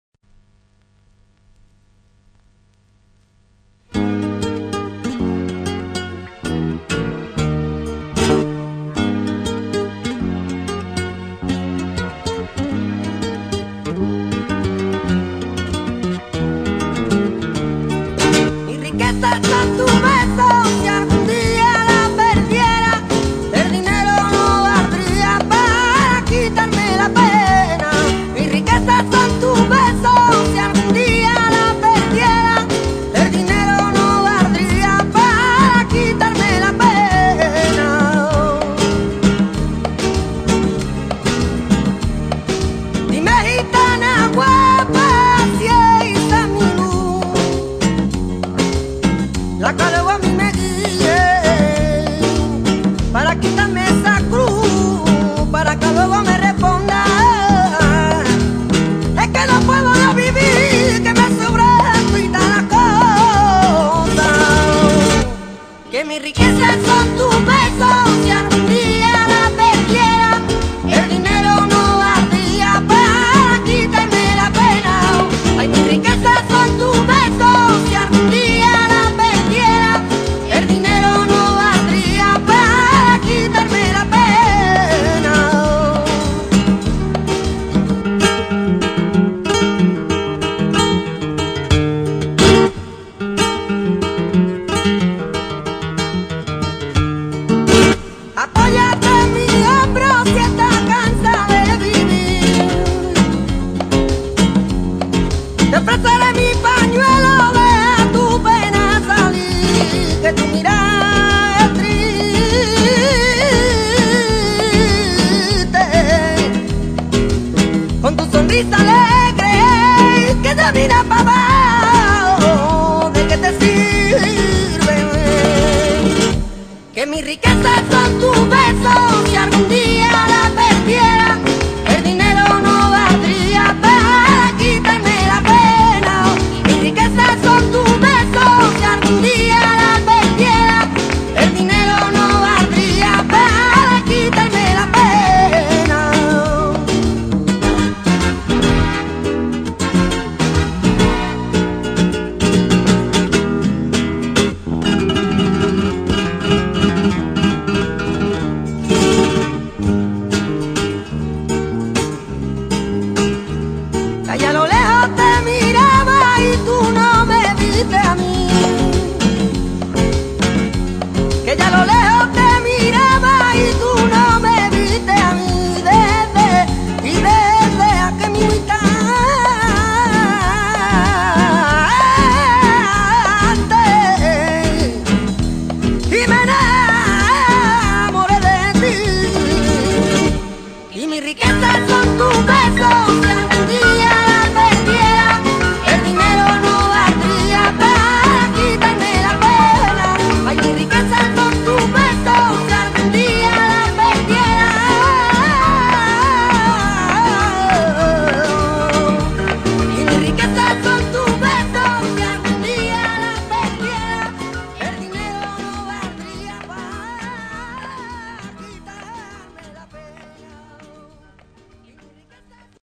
DUQUENDE NOS CANTA POR TANGOS
TEXTO: FEDERICO ALONSO PERNÍA. NOS LLEGA DESDE LAS TIERRAS CATALANAS, ESTE JOVEN CANTAOR, SEGUIDOR DE LOS CANTES DE CAMARÓN, "DUQUENDE" QUE NOS CANTA POR TANGOS, CON LA GUITARRA DE MANZANITA.